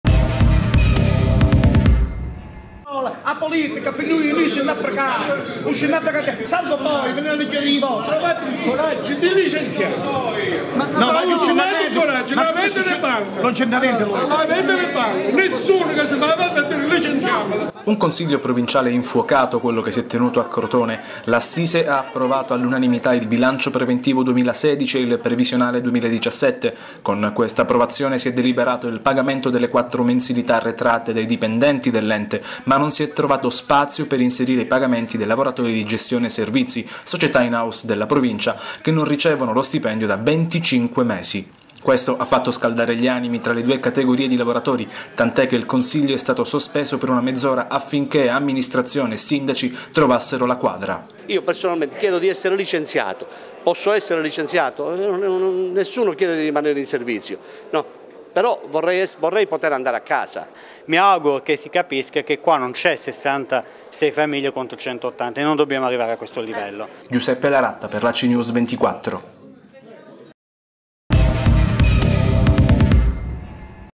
Provincia di Crotone, un Consiglio incandescente
E proprio a Crotone, nella serata di ieri, si è tenuto un consiglio provinciale incandescente. Dopo l’arresto del presidente Parrilla, si fanno i conti con le spettanze del personale.